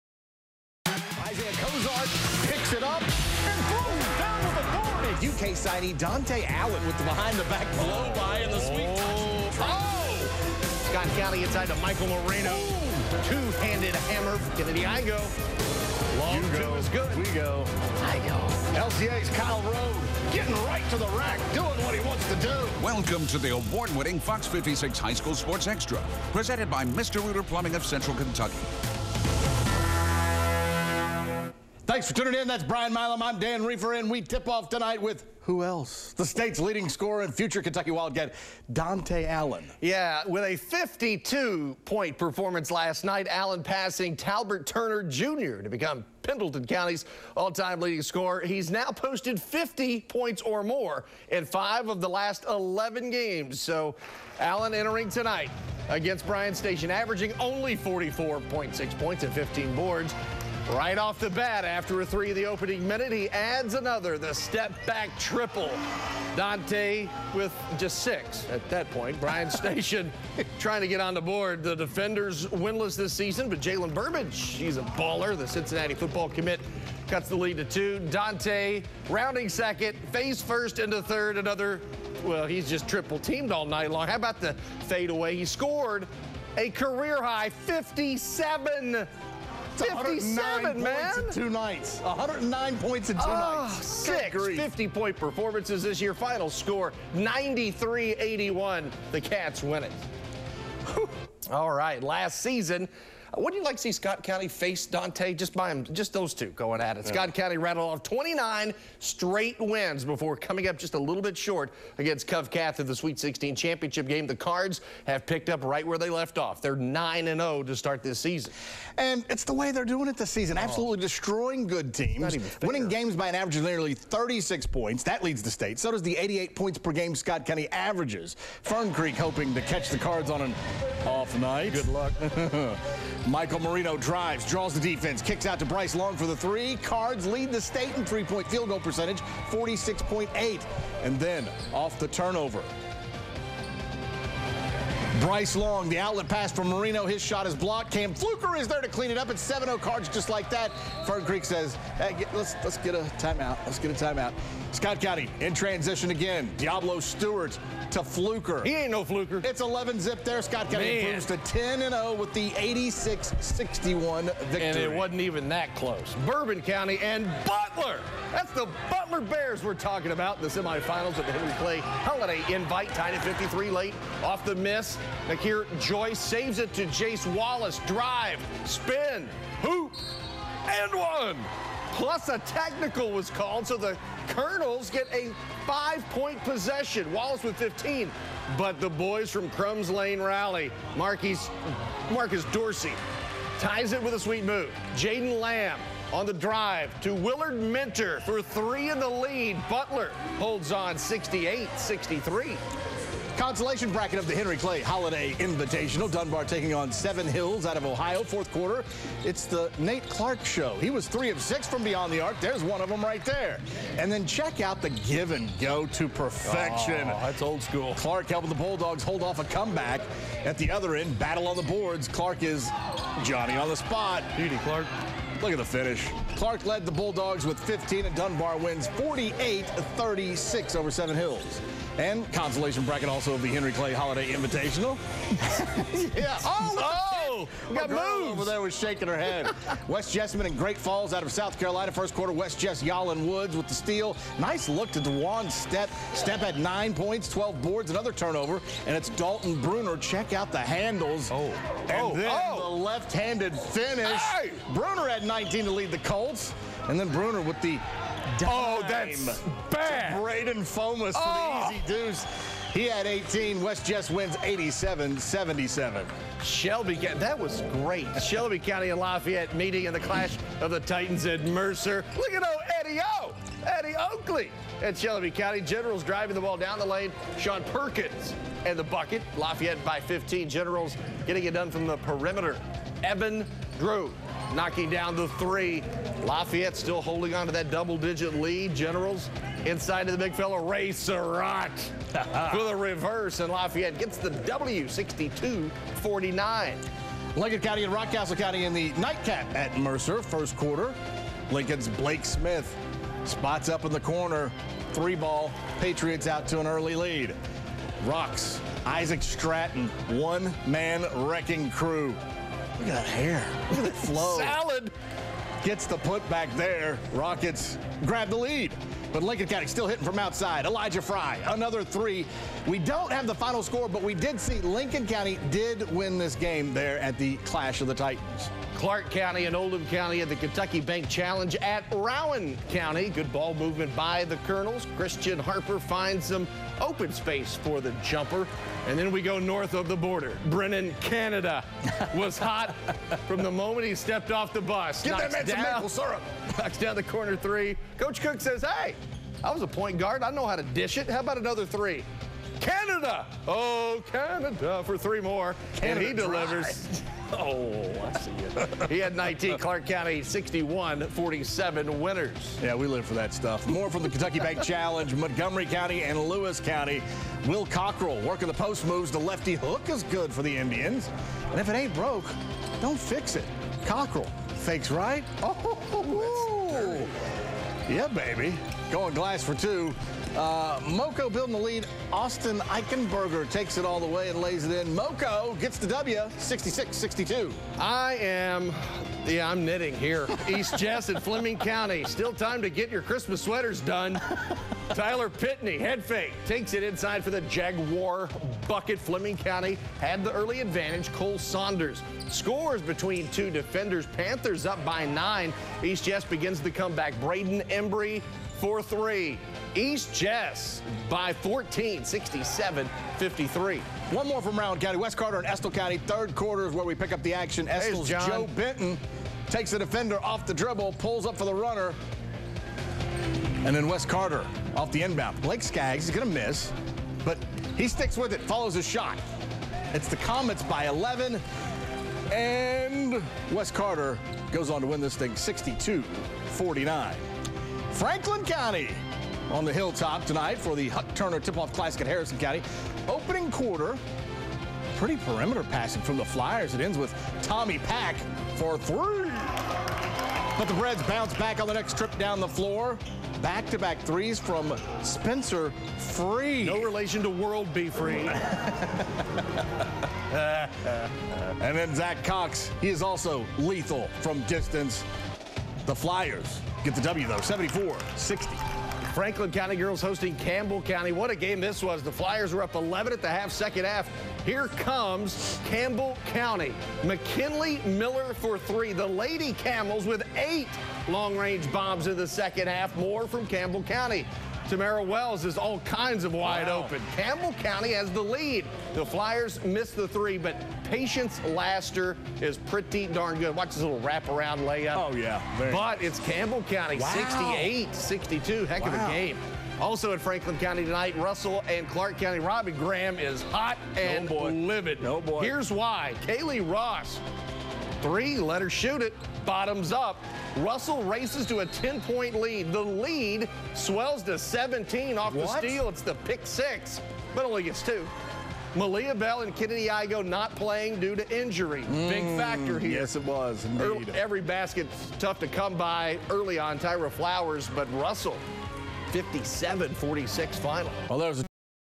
High School Sports Extra - Dec. 21, 2018
Dan Rieffer & Brian Milam with highlights from the following boys' basketball games:
Bryan Station at Pendleton County
Fern Creek at Scott County
Bourbon County vs. Butler
Dunbar vs. Seven Hills, OH
West Jessamine vs. Great Falls, SC
Shelby County vs. Lafayette
Lincoln County vs. Rockcastle County
Clark County vs. Oldham County
Montgomery County vs. Lewis County
Fleming County vs. East Jessamine
Estill County vs. West Carter
Franklin County at Harrison County
Girls' games:
Campbell County at Franklin County
Russell vs. Clark County